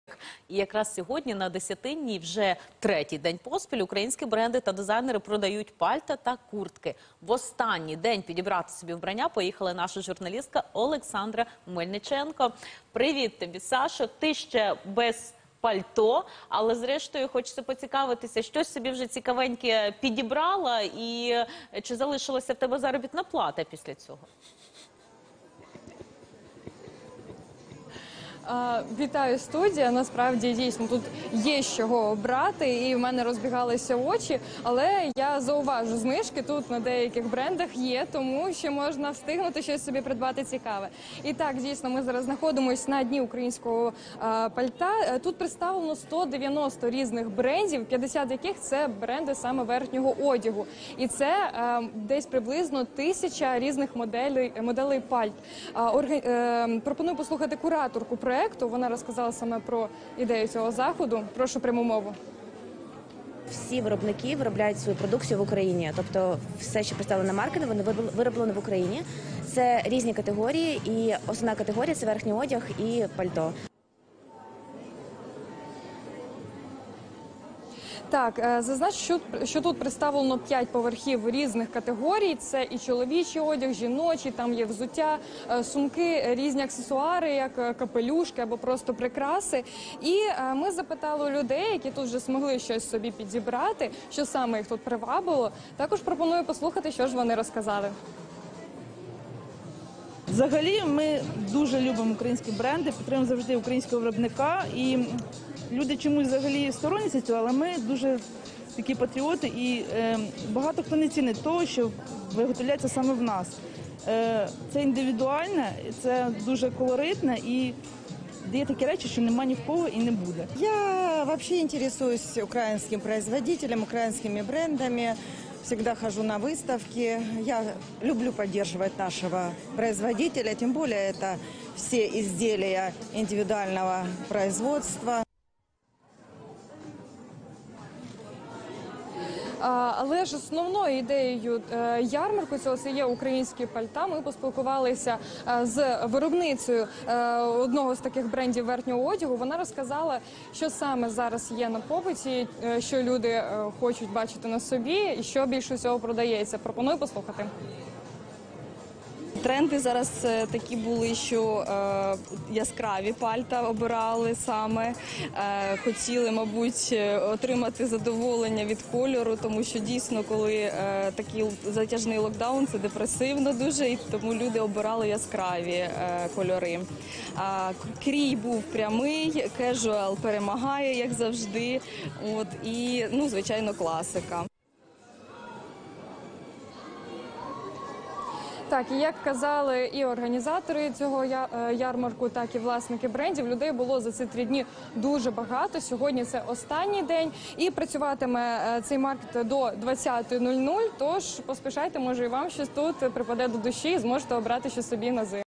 Дні українського пальто у Києві: коли та де?
Цілих три дні на Десятинній, 12, проходитимуть Дні українського пальто від Всі. Свої. Сто українських брендів зберуться під одним дахом і представлять верхній одяг, взуття, сумки, білизну, прикраси та аксесуари. Головний акцент, звісно, буде зроблено саме на пальто. Організатори обіцяють моделі і класичні моделі, а також трендовий оверсайз, пальто з вовни, кашеміру й екошкіри. Вхід безкоштовний.